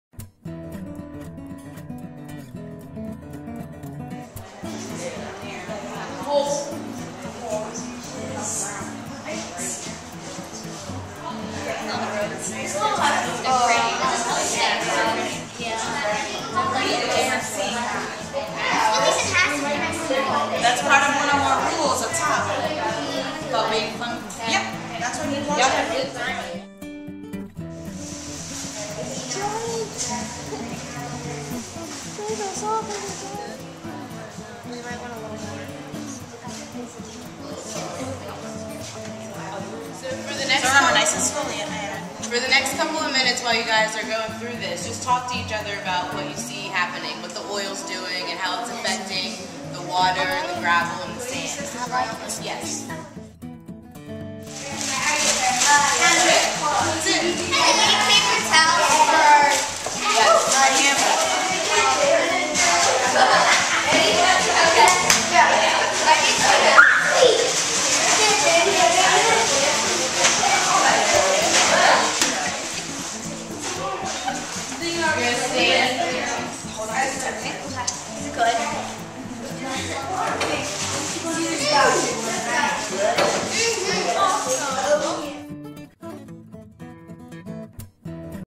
Girls in Gear 2010
Day camp to empower girls through technology jointly run by YWCA Delaware, Girl Scouts of Chesapeake Bay and Girls, Inc.